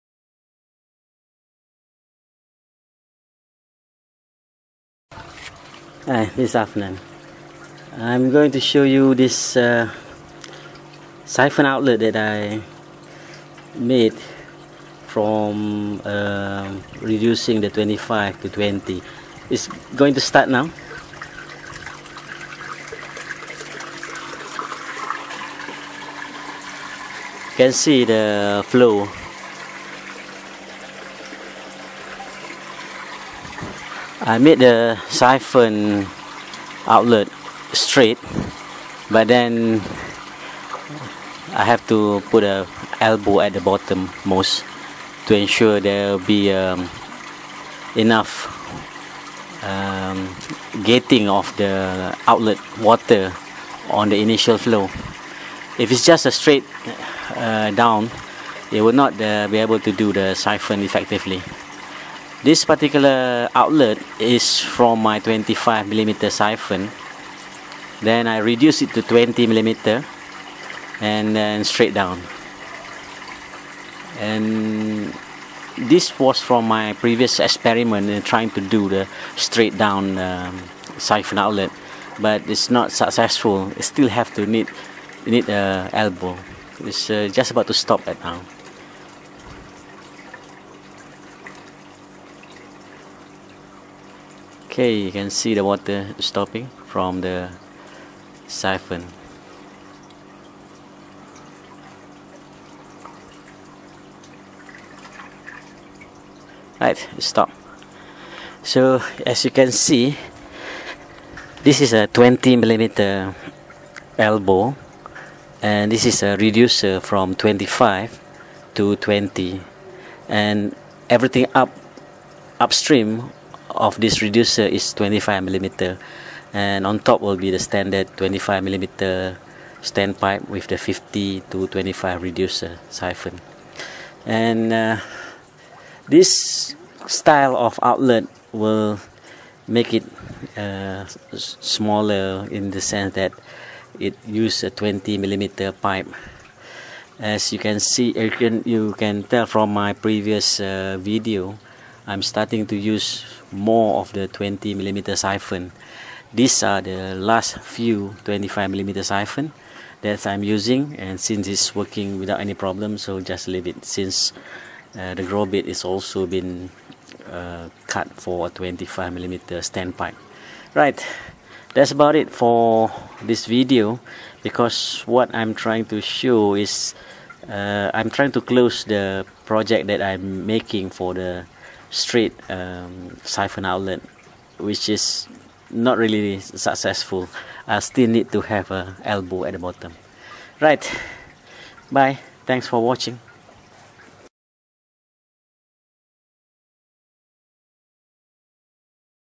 Affnan's Aquaponics - 25mm to 20mm Reducer Outlet
This is a closure video of my Straight Outlet that I was experimenting few months ago. It was tested extensively and I find that though it work but it requires more water flow to start. This will make it dependent on water inflow.

The outlet in this video make use of a 25mm to 20mm reducer with a long neck to an elbow at bottom most of the pipe. This elbow form a water gate pulsing to emptied the siphon bell causing flush at lower water inflow as compared to straight flow outlet.